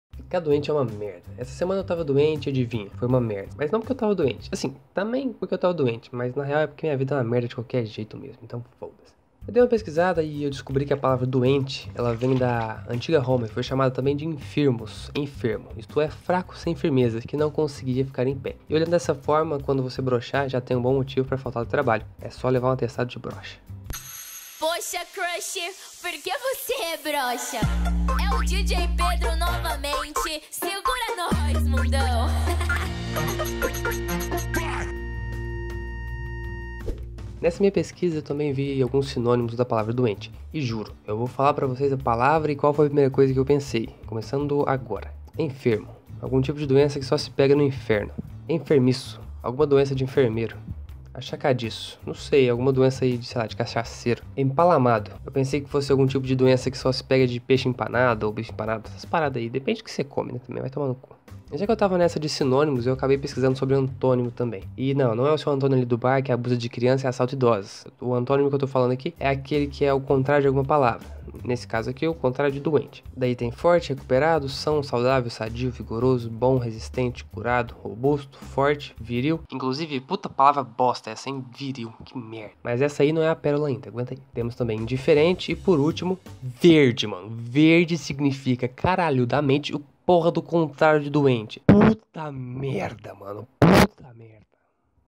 FICAR DOENTE 😣
🔵Nesse rápido vídeo rola algumas piadas sobre ficar doente e essas paradas relacionadas...

☎️Me encontre por aí☎️

💢Conheça também💢

✔Compartilhe 📢